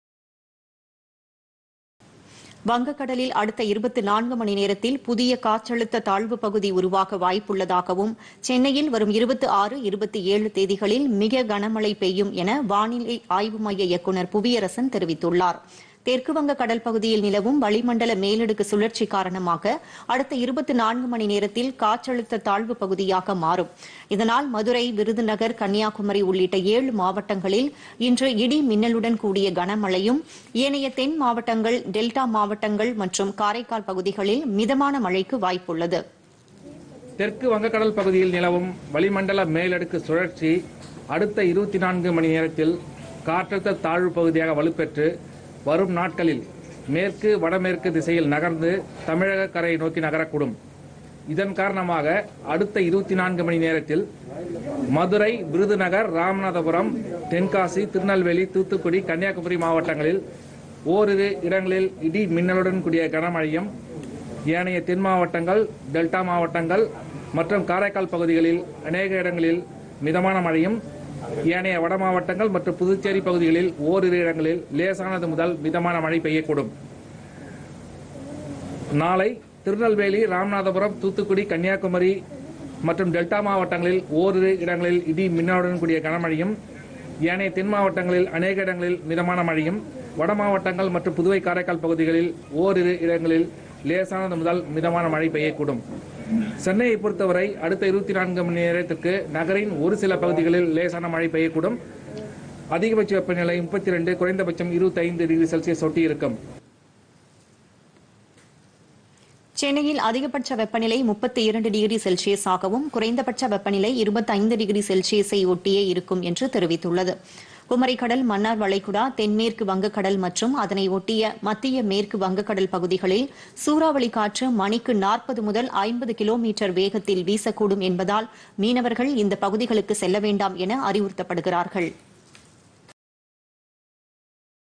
புதிய காற்றழுத்த தாழ்வு பகுதிPodhigaiTamilNews
வங்கக்கடலில்  அடுத்த 24 மணிநேரத்தில் புதிய காற்றழுத்த தாழ்வு  பகுதி - சென்னையில் வரும் 26, 27  தேதிகளில் மிக கன மழை பெய்யும் - வானிலை ஆய்வு மைய இயக்குநர் புவியரசன் தகவல்